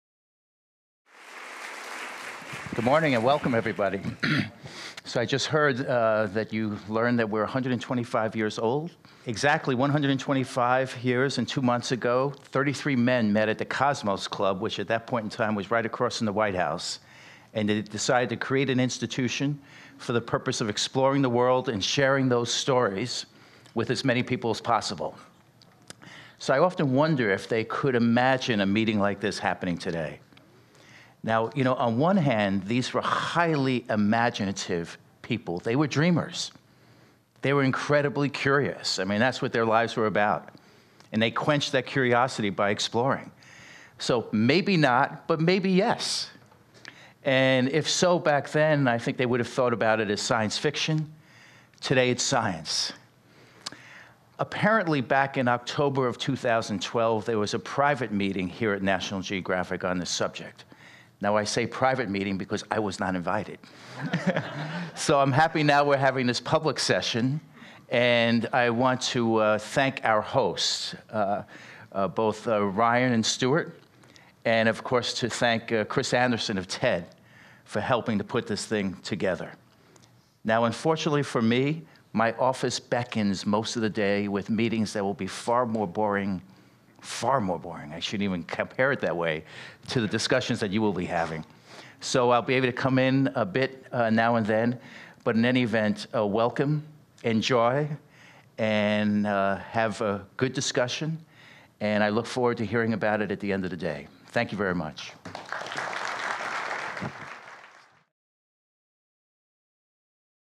A New Century of Exploration: John Fahey at TEDxDeExtinction
John Fahey was appointed chairman and chief executive officer of the National Geographic Society in January 2011. He served as president and chief executive officer of the Society from March 1998 to December 2010 and as chairman of the executive committee of its board of trustees. During his tenure Fahey has led an evolution of the National Geographic Society, including its entry into cable television with the National Geographic Channels, and the international expansion of National Geographic magazine.

To learn more about de-extinction, please visit Revive & Restore (the organizer of TEDxDeExtinction)